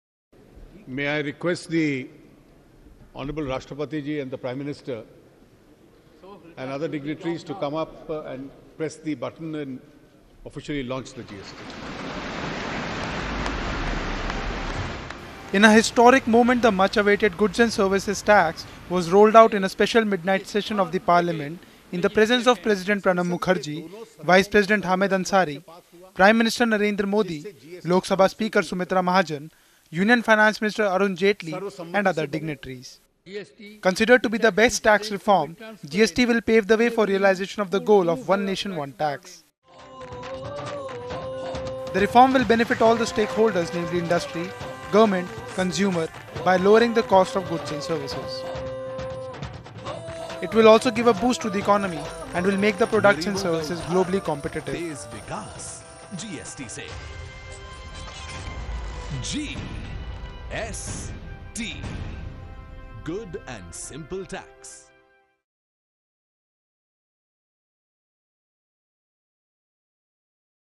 GST rollout : PM & President officially launch GST | Oneindia News
The much-awaited and much-hyped Goods and Services Tax (GST) was rolled out in a special midnight session of the Parliament, in the presence of President Pranab Mukherjee, Vice President Hamid Ansari, Prime Minister Narendra Modi, Lok Sabha Speaker Sumitra Mahajan, Union Finance Minister Arun Jaitley and other dignitaries. A grand event was organised for this historical launch.